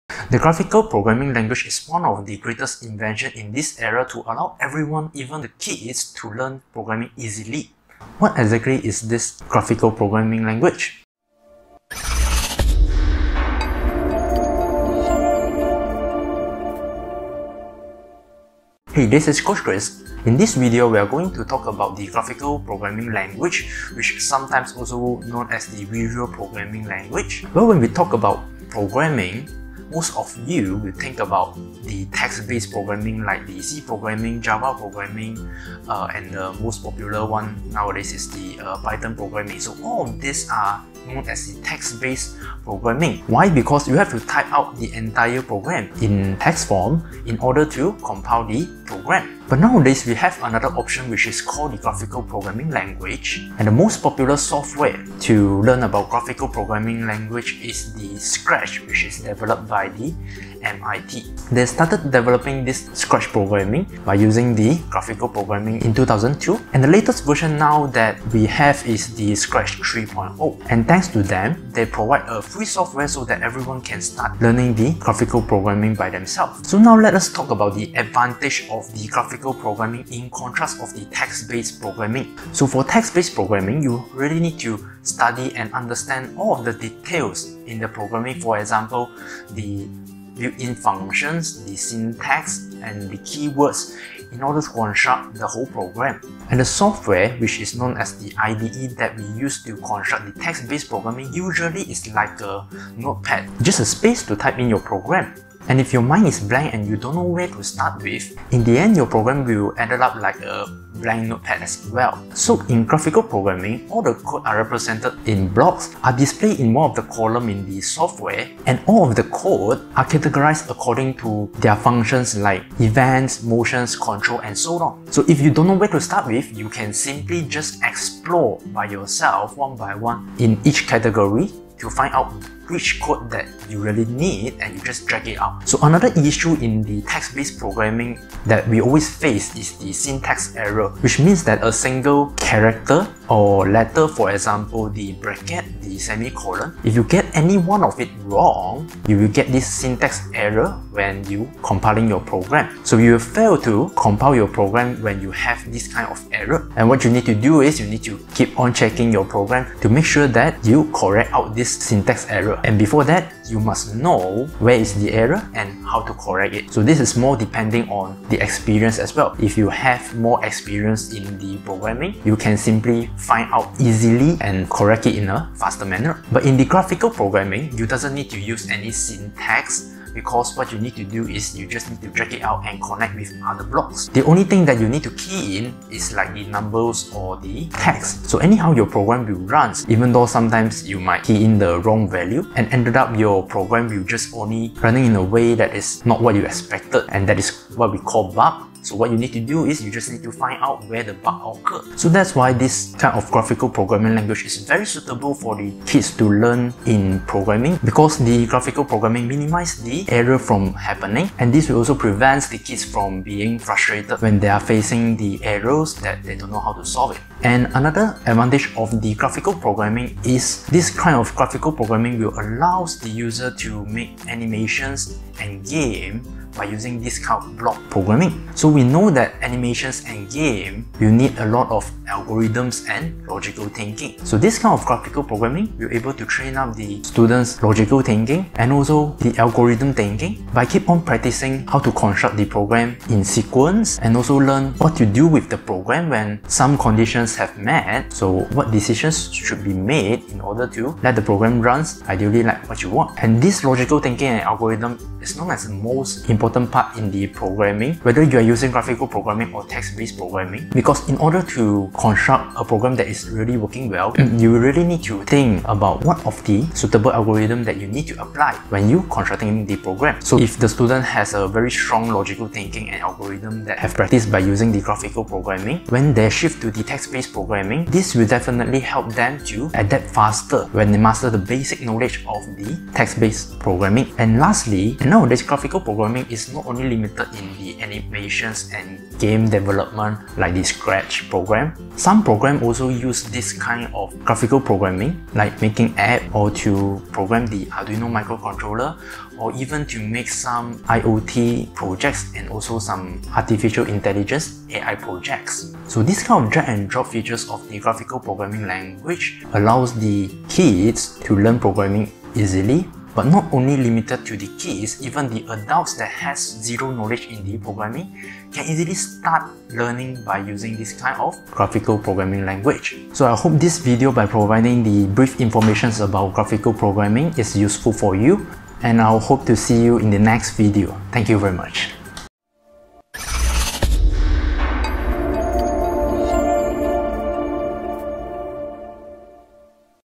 Graphical Programming Language: The Simplest Programming Language
In this video, we introduce the programming language that everyone can learn - The Graphical Programming Language.

We proudly introduce our online courses now:
-MBlock Basic Programming Class
-Arduino Basic C Programming Class